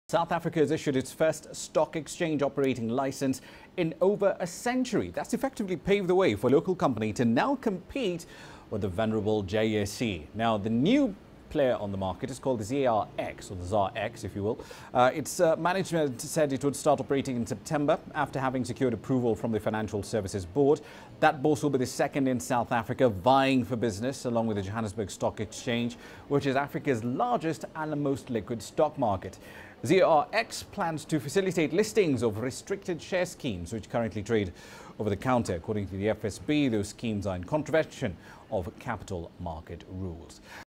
JSE grants first bourse licence in over 100 years
South Africa has issued its first stock exchange operating licence in over 100 years. That's paved the way for a local company to compete with the Johannesburg Stock Exchange. The ZAR X Stock Exchange said it would start operating in September after securing approval from the Financial Services Board.